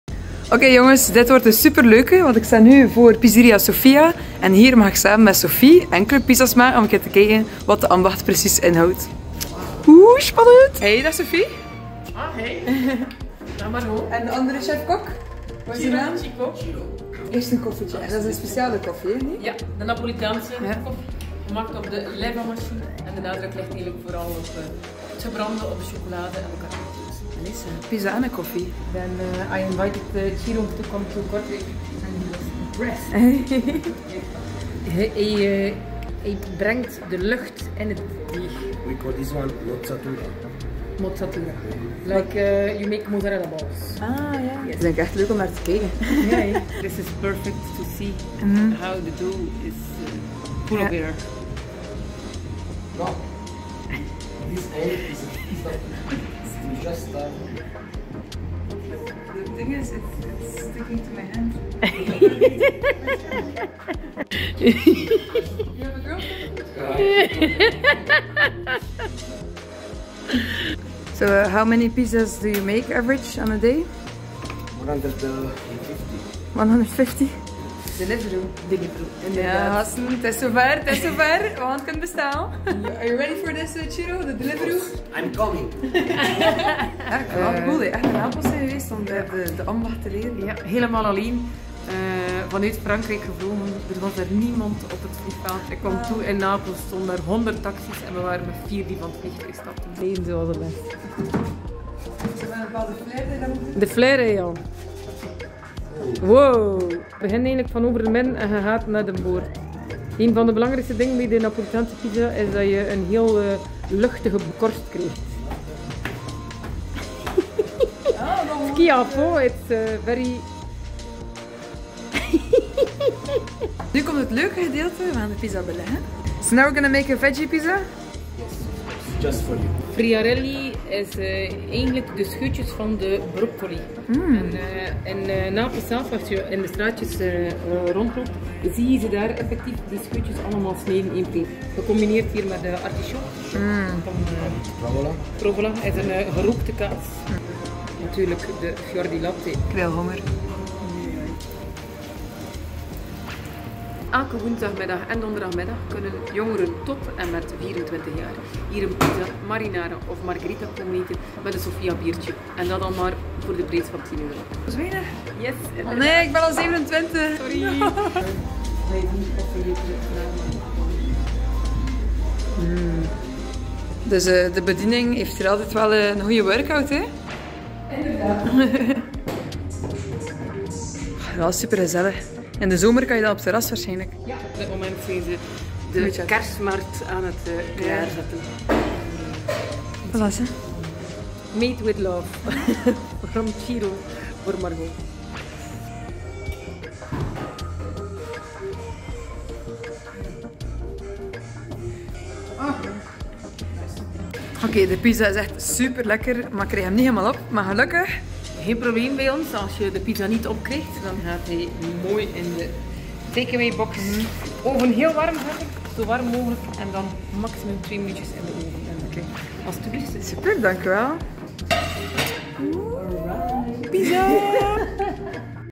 Getest & Goedgekeurd: Pizzeria Sofia
Pizzeria Sofia staat voor echte Napolitaanse Pizza met de female touch van Sofie Delbeke. Begin 2021 ging ze een nieuwe uitdaging aan, Sofie boekte een vlucht naar Napels en volgde er een opleiding van drie weken in de Associazione Verace Pizza Napoletana Academy. Daar leerde ze authentieke Napolitaanse pizza’s maken.

Enkele Napolitanen kwamen speciaal naar Kortrijk om er steen per steen een Napolitaanse oven te bouwen. Meer nog, Sofie zet ook twee ervaren Italiaanse pizzakoks in. Ik had het genoegen om kennis te maken met de charmante Ciro 😊

Op woensdag- en donderdagmiddag genieten jongeren t.e.m. 24 jaar van een leuke deal: 1 margherita of marinara met Sofia biertje aan 10 euro!

Volg de ambachtelijke pizza’s van Pizzeria Sofia op Instagram: @pizzeria_sofia_by_beudaert

Pizzeria Sofia is open van woensdag t.e.m. zondag, van 12u tot 14u en van 18u tot 22u. Takeaway mogelijk voor zij met minder tijd om te tafelen in het gezellige kader boven Table d’Amis.

📍 Sint-Maartenskerkhof 8